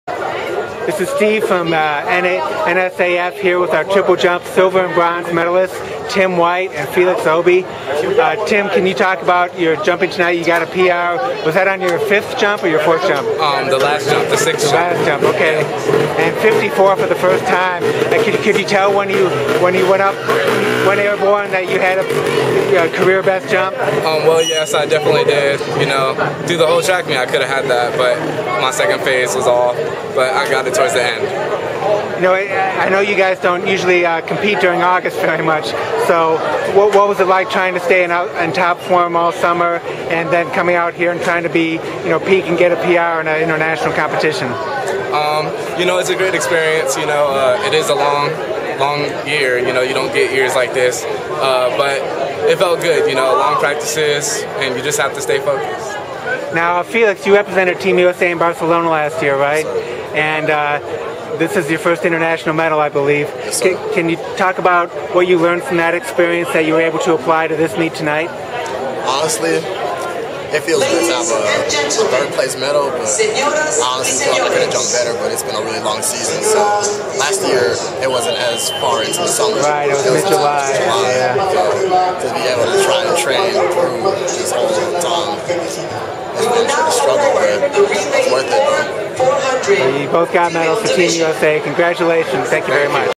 Tim White y Felix Obi USA SILVER y BRONZE triple jump final Pan Am Jrs
Tim White and Felix Obi, Team USA, after winning SILVER and BRONZE in the men's triple jump at the 2013 Pan American Junior Championships.